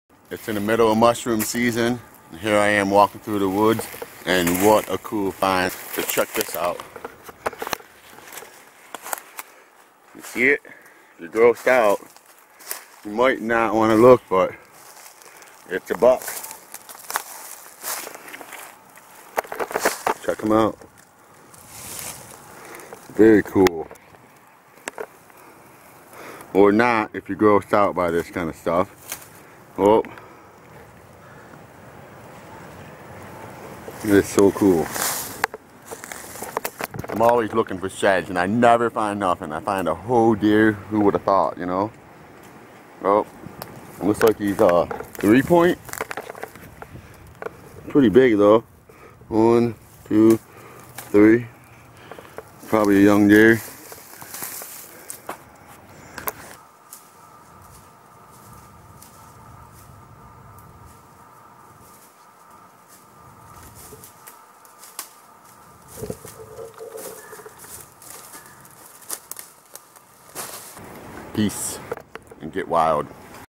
Found Dead Buck In the Woods While Hunting for Morel Mushrooms
Found a 3pt buck dead in the woods. antlers were very cool looking so I kept them...